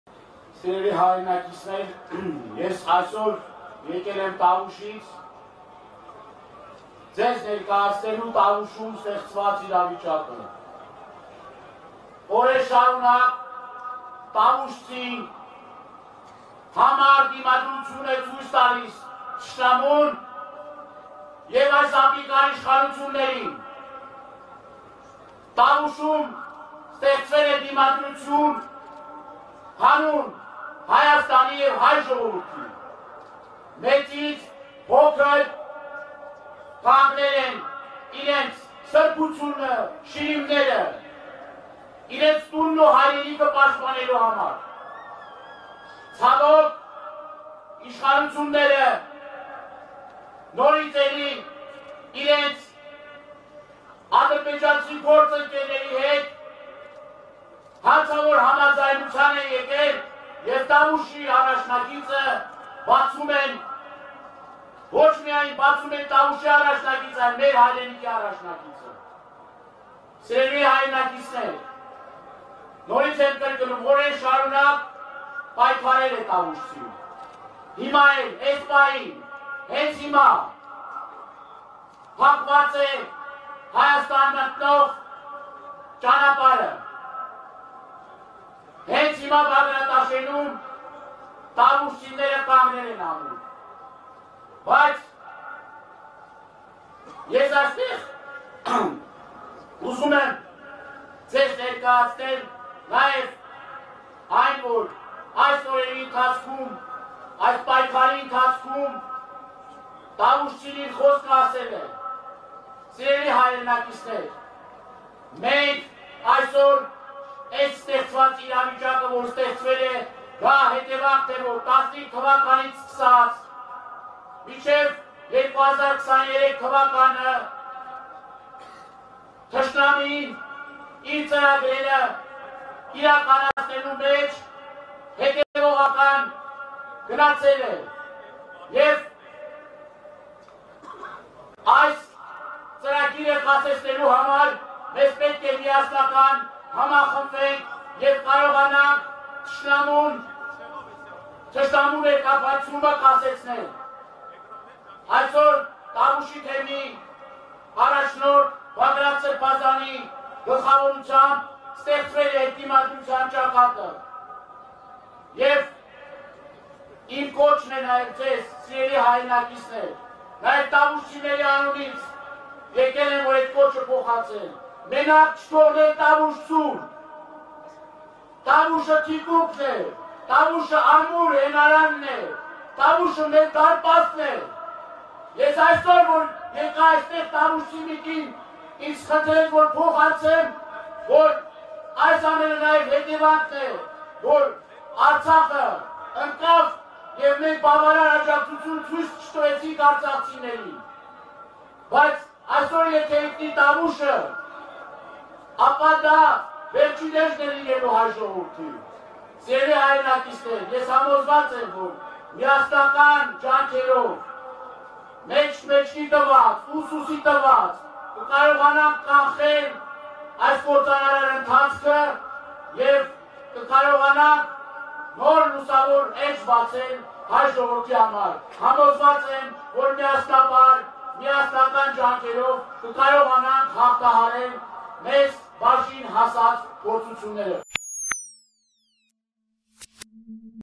Մենակ չթողնեք տավուշցուն,Արցախը կորցրինք` չկարողանալով համախմբվել, Տավուշը մեր դարպասն է. Դանիելյան
Տավուշում ստեղծվել է Հայրենիքի պաշտպանության ճակատ Տավուշի թեմի առաջնորդ Բագրատ Սրբազանի առաջնորդությամբ ու տավուշցին կանգնել է պաշտպանելու իր հողը․ պատգամավոր